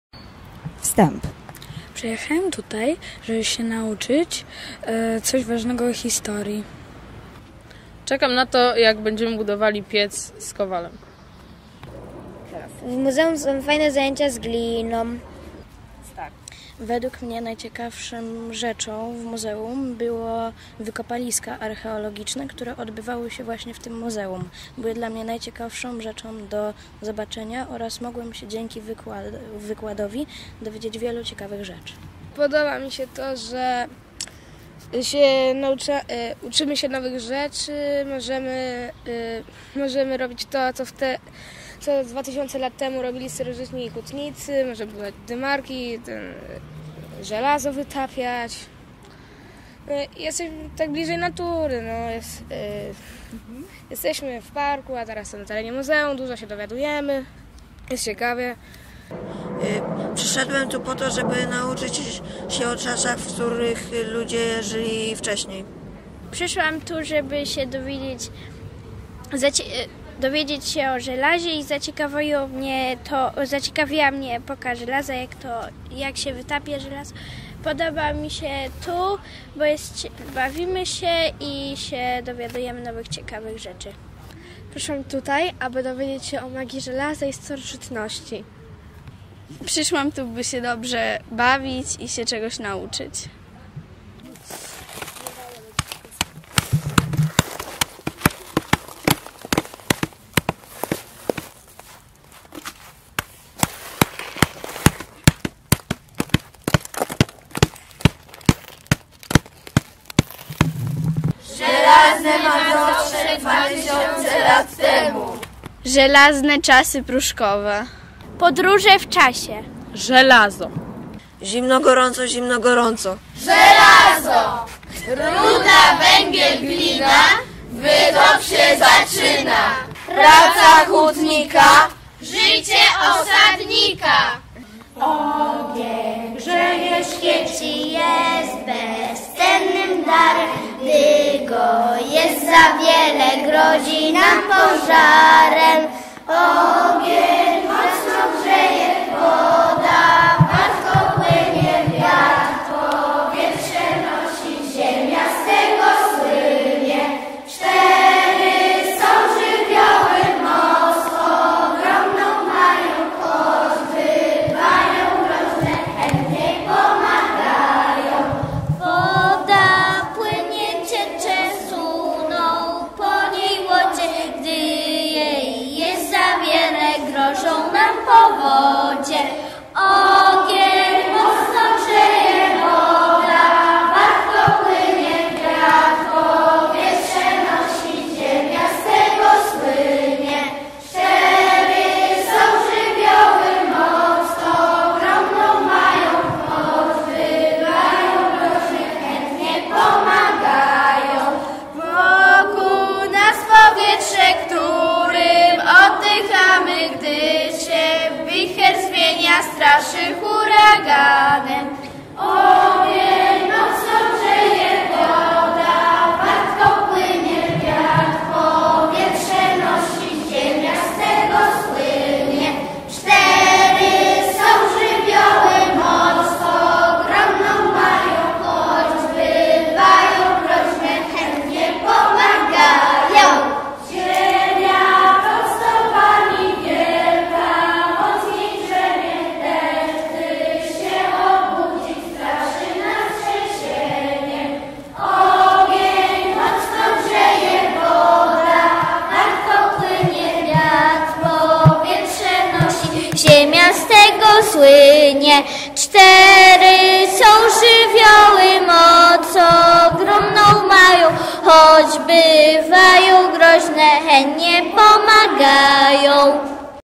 Wstęp - Warsztaty Historyczno-Artystyczne Czas Żelaza 2017
Relacja z przebiegu Warsztatów Historyczno-Artystycznych "Czas Żelaza" organizowanych przez Muzeum Starożytnego Hutnictwa Mazowieckiego im. Stefana Woydy w Pruszkowie w dniach: 26-30 czerwca 2017.